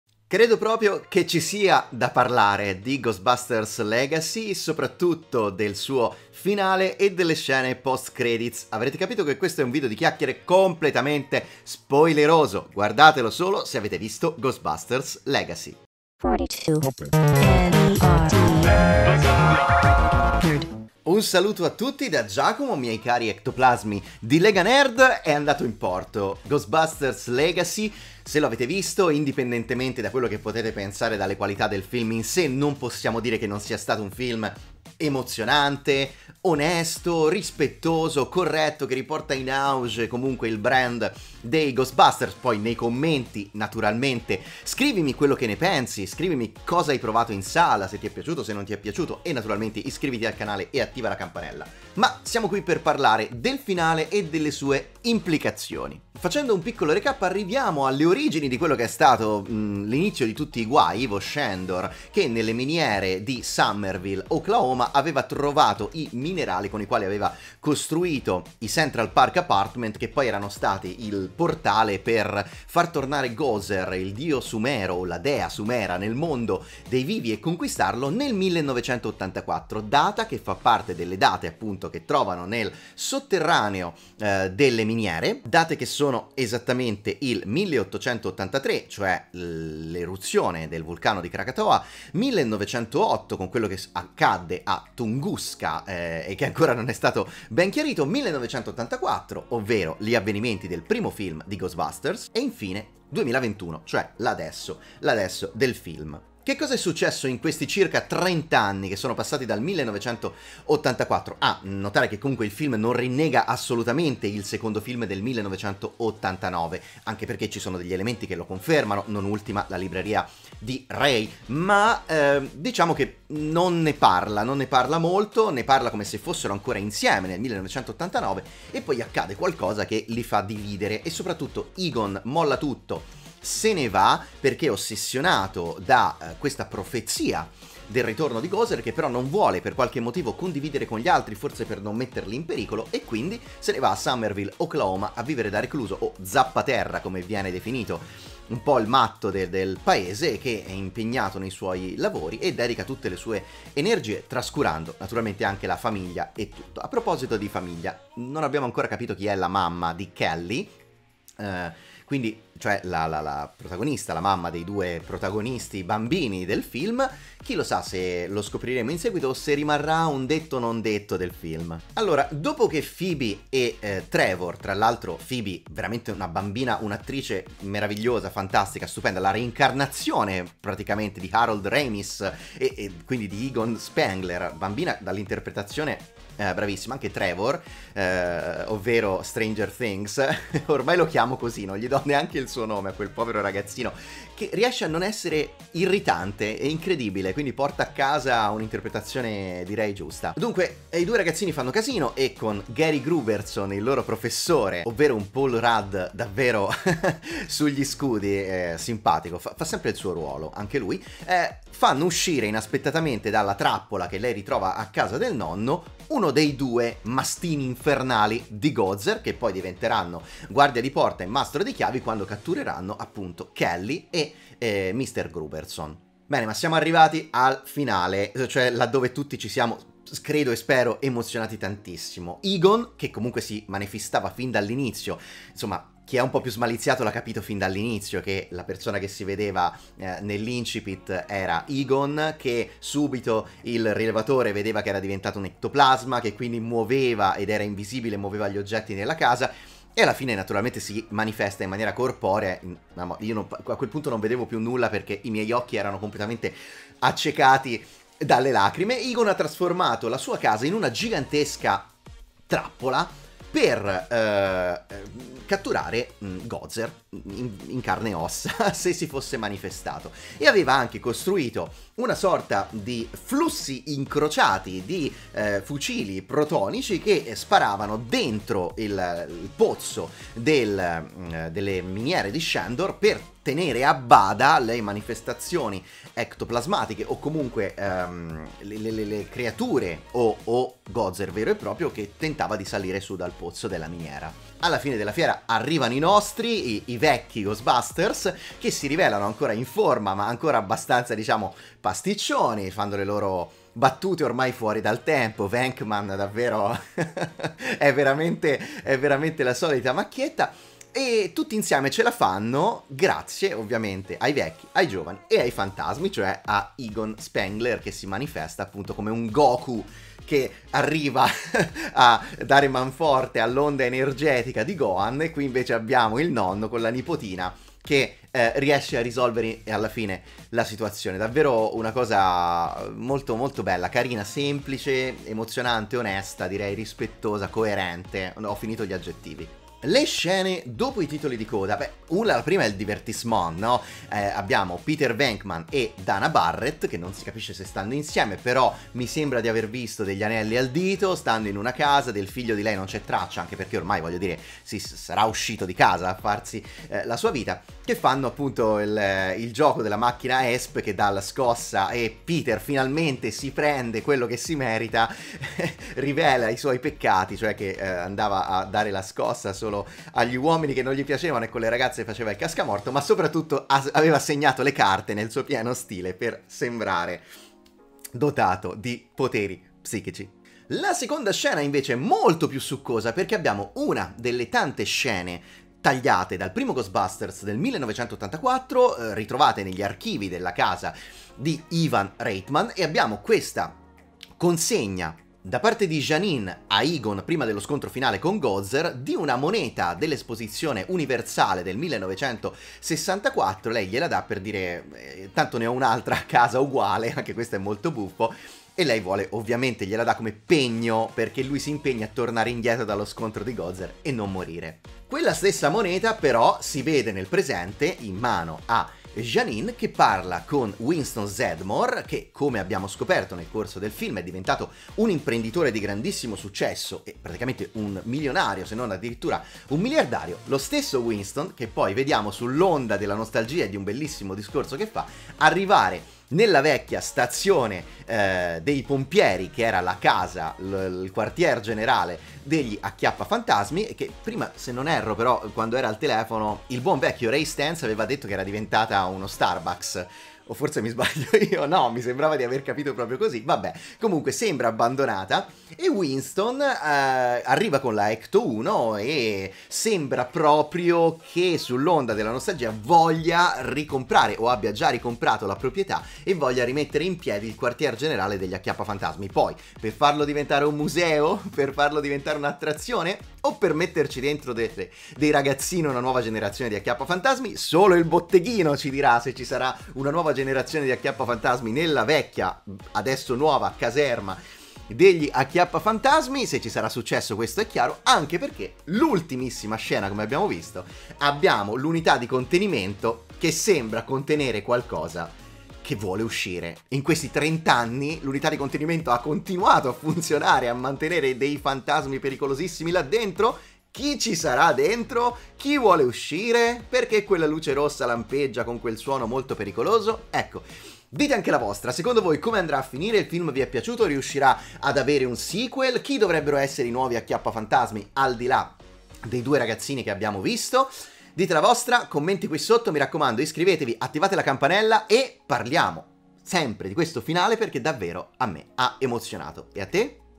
Ghostbusters Legacy, Spiegazione del Finale e delle Scene Post Credits
Parliamo del finale di Ghostbusters Legacy e delle scene dopo i titoli di coda: e tu cosa ne pensi?